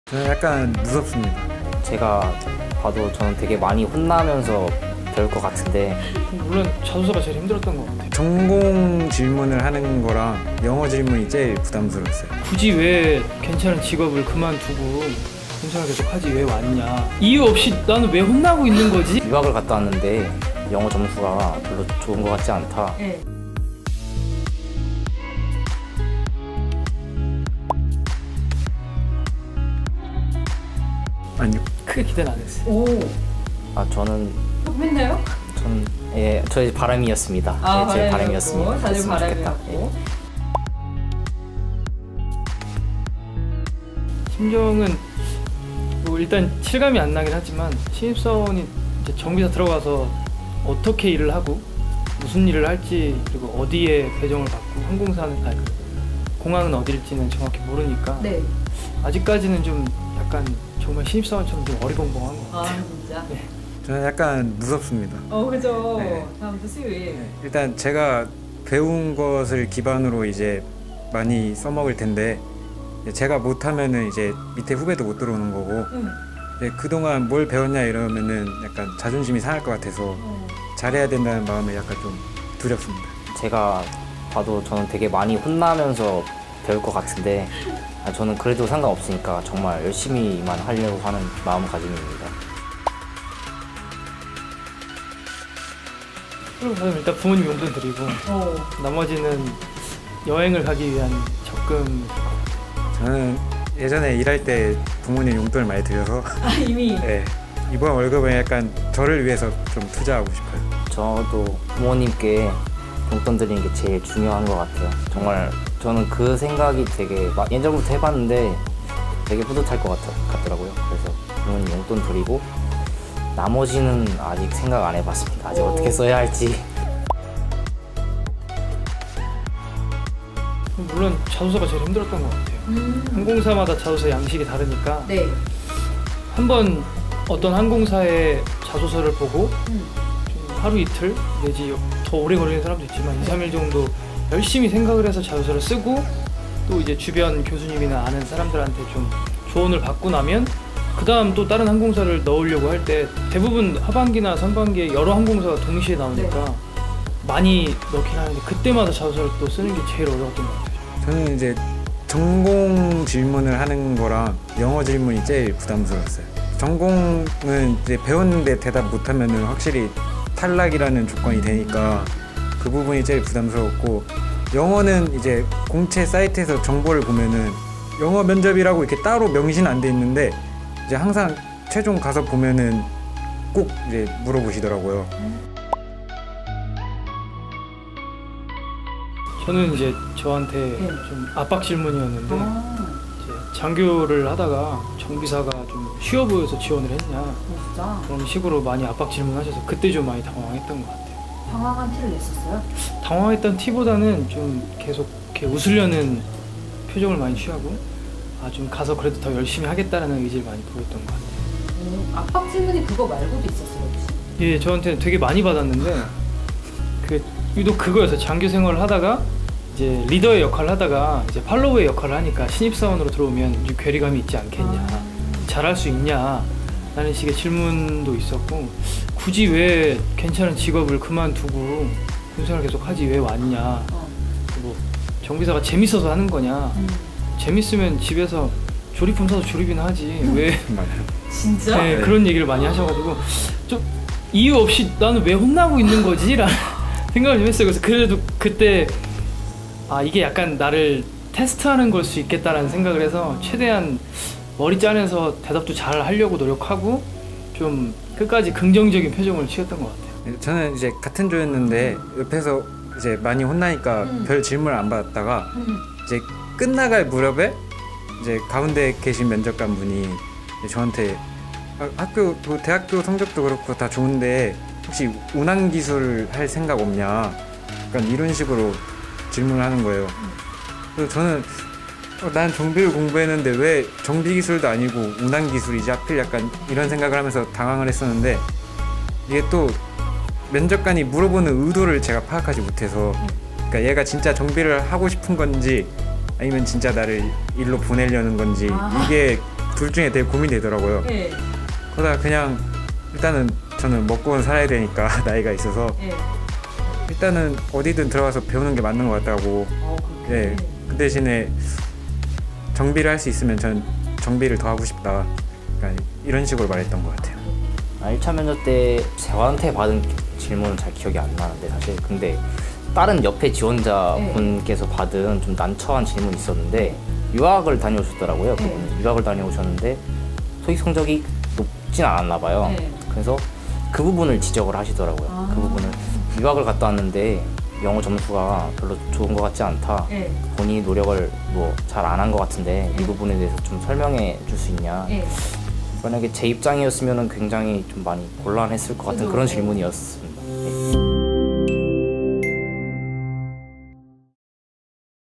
티웨이 항공 면접 질문이 궁금하시다구요?? │ 과기전TV
티웨이 항공 합격자들이 알려주는 면접 이야기!
압박 질문도 나왔다는데..ㄷㄷㄷ